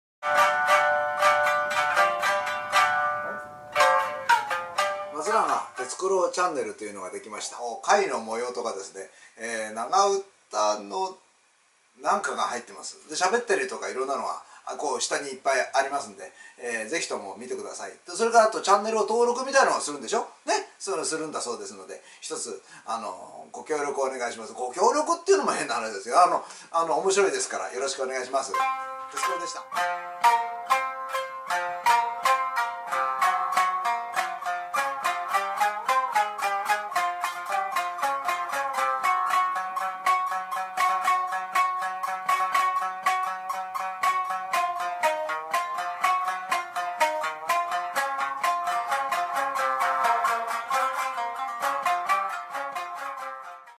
「鉄九郎チャンネル」へようこそ
YouTubeに、長唄三味線方、松永鉄九郎さんの動画を集めたチャンネルを作りました。
しゃべる、しゃべる、しゃべる、「長唄の会」のトーク集、本日の一言、稽古をつける、演奏する、作曲集も……
そこには、鉄九郎さんの情熱も長唄の何か…愛かな　も入っています。